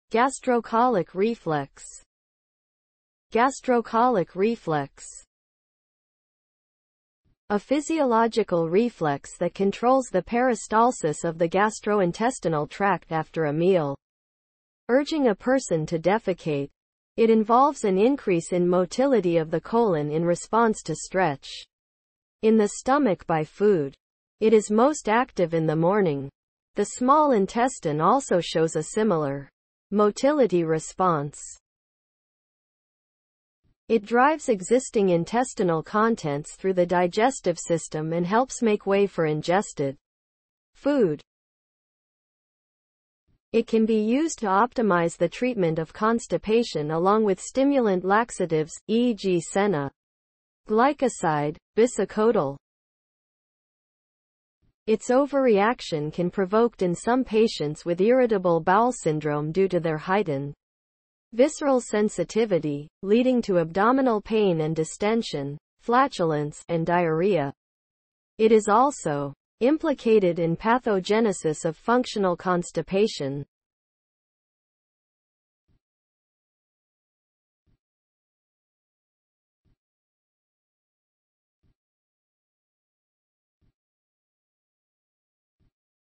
gastrocolic reflex
(gastrocolic response)

A physiological reflex that controls the peristalsis of the gastrointestinal tract after a meal, urging a person to defecate. It involves an increase in motility of the colon in response to stretch in the stomach by food. It is most active in the morning.
The small intestine also shows a similar motility response.

It drives existing intestinal contents through the digestive system and helps make way for ingested food.

It can be used to optimise the treatment of constipation along with stimulant laxatives (e.g. senna glycoside, bisacodyl).

Its overreaction can provoked in some patients with irritable bowel syndrome due to their heightened visceral sensitivity, leading to abdominal pain and distension, flatulence, and diarrhea.
It is also implicated in pathogenesis of functional constipation.